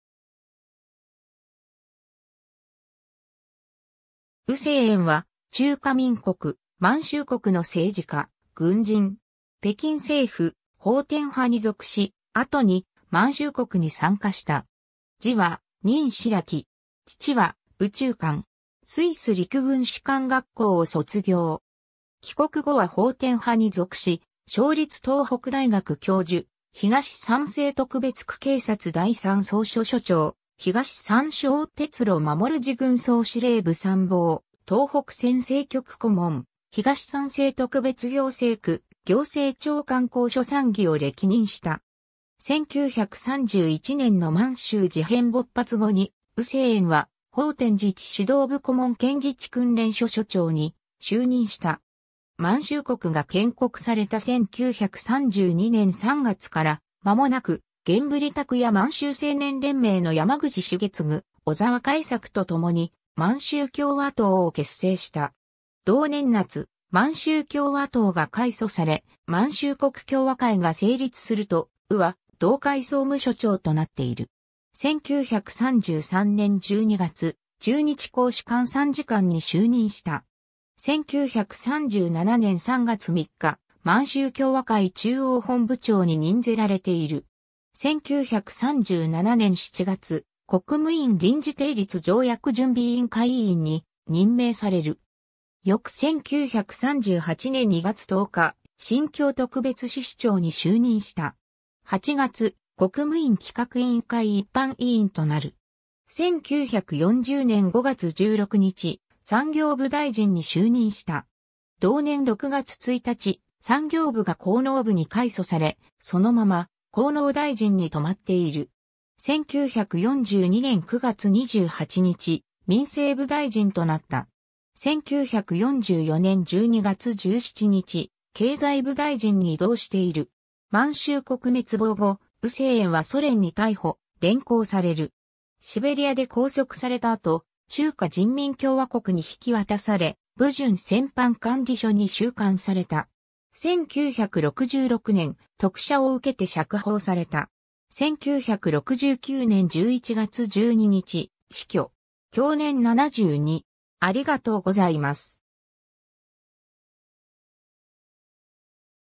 于静遠
于 静遠（う せいえん）は中華民国、満州国の政治家・軍人。
北京政府、奉天派に属し、後に満州国に参加した。
字は任樸。
父は于沖漢。
 スイス陸軍士官学校を卒業。
帰国後は奉天派に属し、省立東北大学教授、東三省特別区警察第三総署処長、東三省鉄路護路軍総司令部参謀、東北船政局顧問、東三省特別行政区行政長官公署参議を歴任した。
 1931年（民国20年）の満州事変（九・一八事変）勃発後に、于静遠は奉天自治指導部顧問兼自治訓練処処長に就任した。
満州国が建国された1932年（大同元年）3月からまもなく、阮振鐸や満州青年連盟の山口重次・小澤開作とともに満州協和党（後の満州国協和会）を結成した。
同年夏、満州協和党が改組され、満州国協和会が成立すると、于は同会総務処長となっている。
1933年12月、駐日公使館参事官に就任した。
1937年（康徳4年）3月3日、満州協和会中央本部長に任ぜられている。
 1937年（康徳4年）7月、国務院臨時訂立条約準備委員会委員に任命される。
翌1938年（康徳5年）2月10日、新京特別市市長に就任した。
8月、国務院企画委員会一般委員となる。
1940年（康徳7年）5月16日、産業部大臣に就任した。
同年6月1日、産業部が興農部に改組され、そのまま興農大臣に留まっている。
1942年（康徳9年）9月28日、民生部大臣となった。
1944年（康徳11年）12月17日、経済部大臣に異動している。
 満州国滅亡後、于静遠はソ連に逮捕、連行される。
シベリアで拘束された後、中華人民共和国に引き渡され、撫順戦犯管理所に収監された。
1966年、特赦を受けて釈放された。
1969年11月12日、死去。
享年72 。